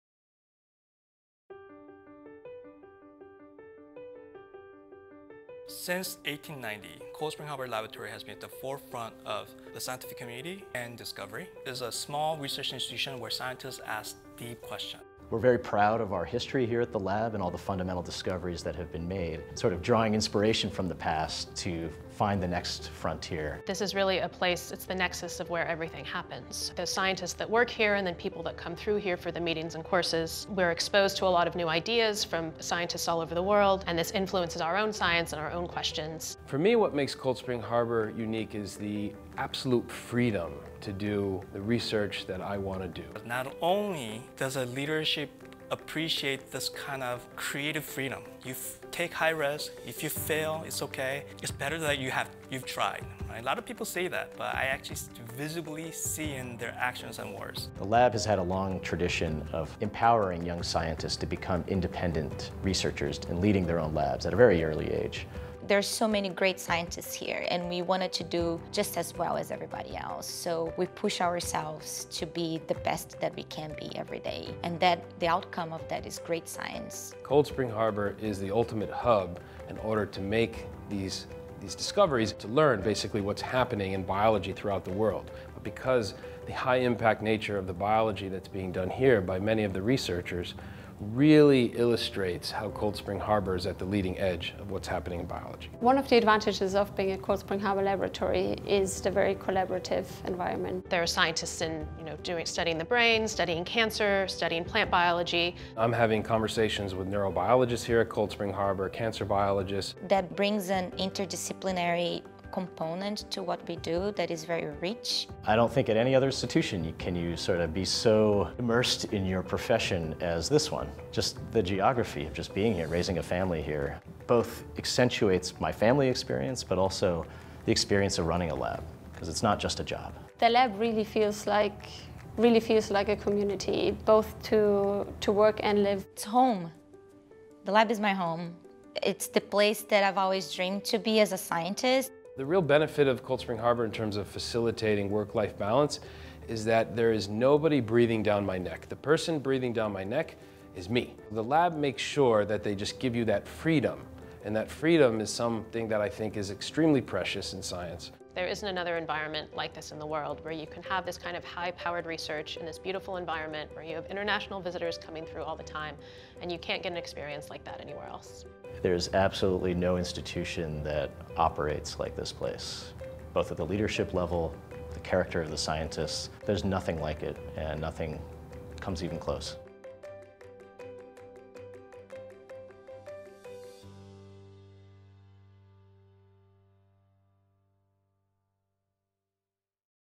A home like no other, Cold Spring Harbor Laboratory
Cold Spring Harbor Laboratory is more than just a place where work is done. Hear why our campus, our community, and our collaborative nature makes us a place that so many scientists call "home."